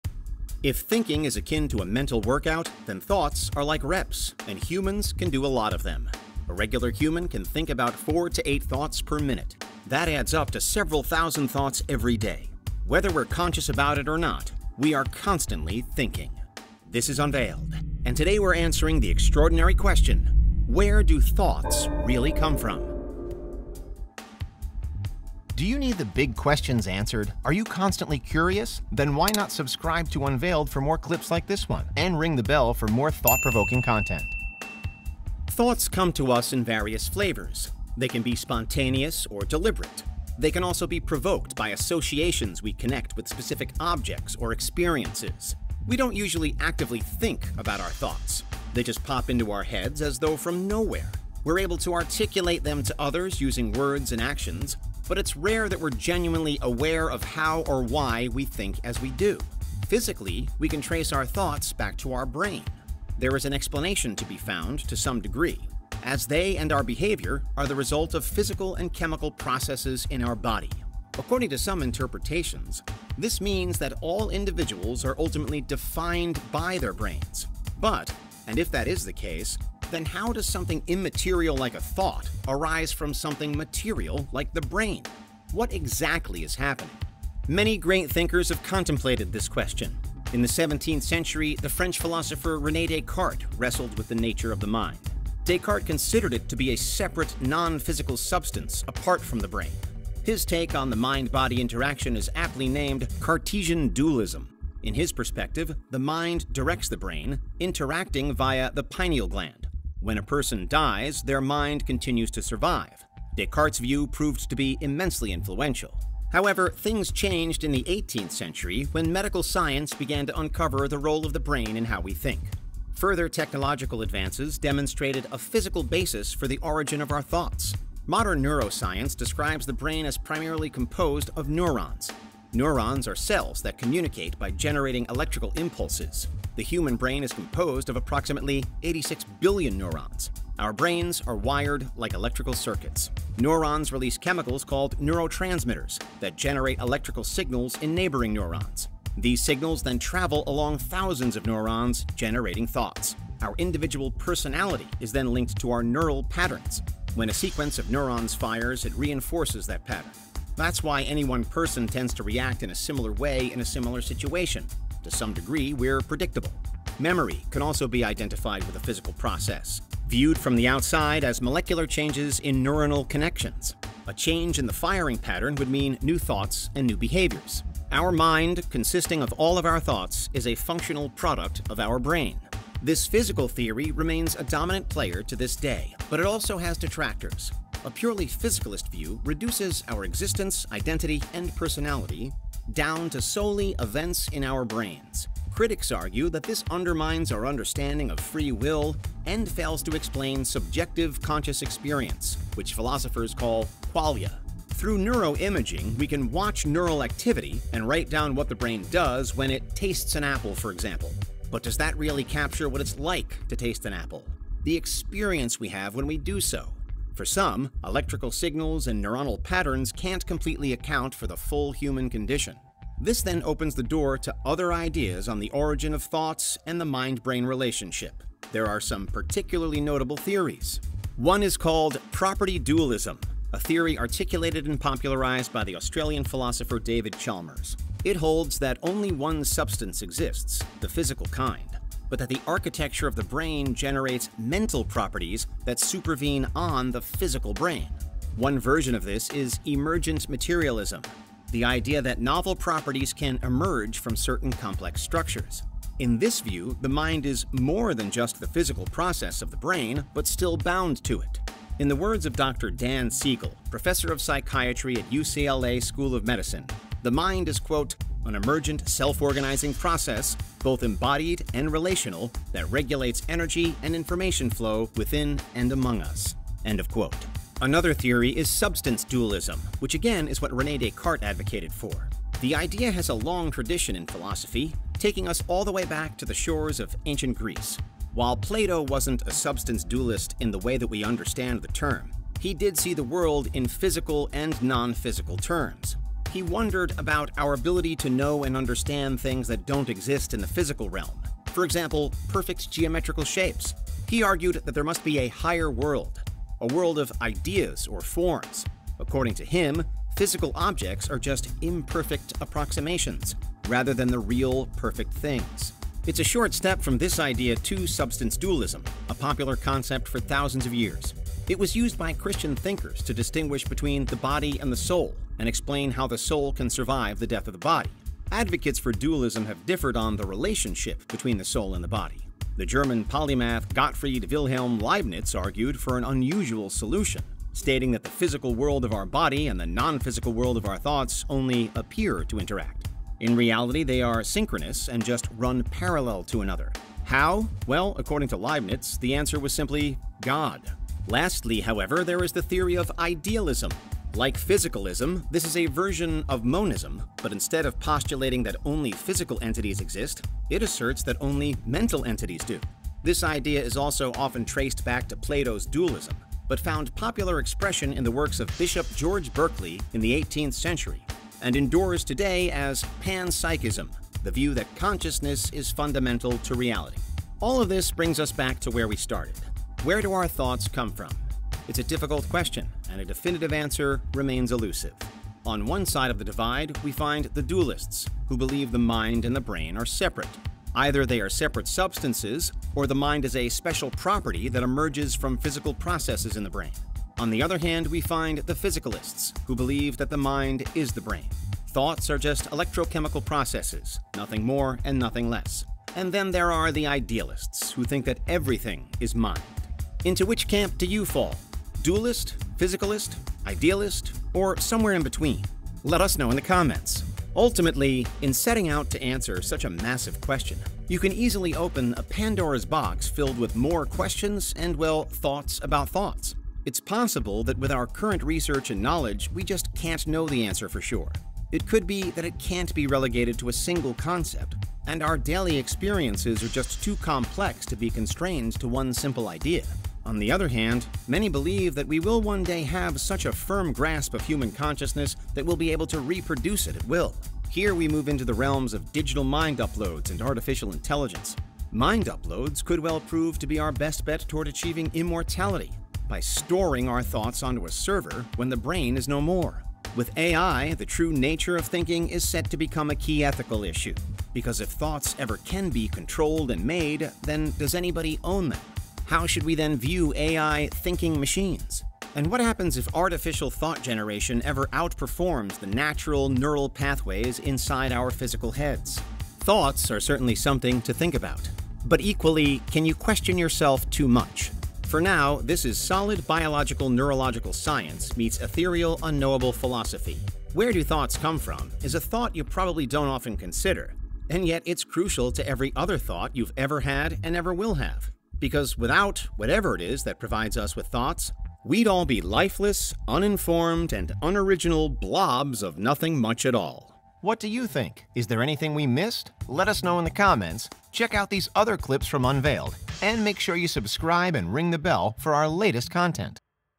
Where Do Thoughts Really Come From? | Unveiled
Where do your thoughts... come from?? Join us... and find out!

In this video, Unveiled takes a closer look at the true science of thinking! We all have hundreds of thoughts all of the time... but how do they really work?